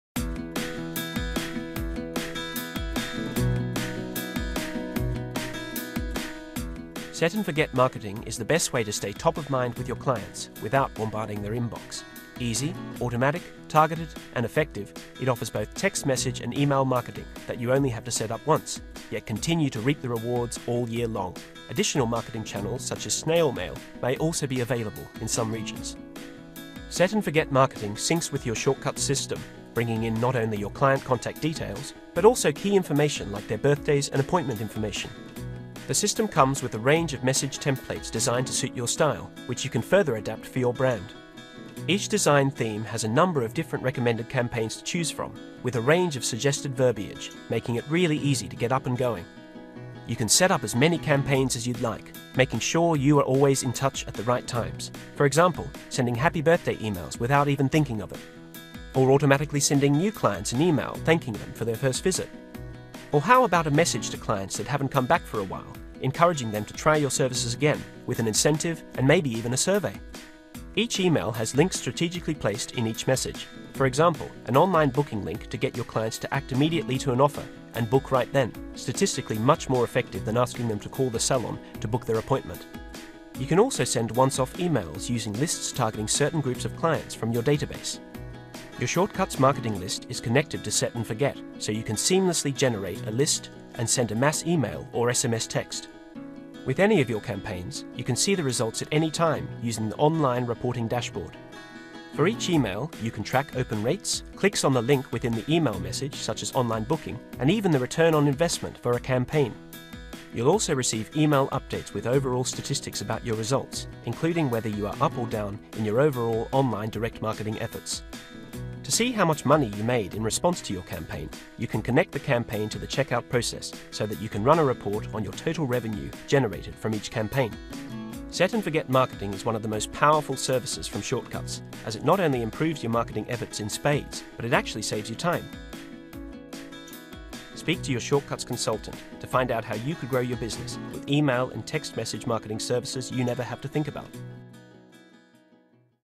Shortcuts Set & Forget Marketing Demo Video
Take the tour of the Shortcuts Software Set & Forget Marketing tools, and find out how they can help you grow your business. Watch this demonstration video to find out how Shortcuts Set & Forget Marketing can help your salon deliver timely, relevant, and targeted messages, with interactive, two-way communication, as well as detailed reports on each campaign, and custom surveys for feedback reporting.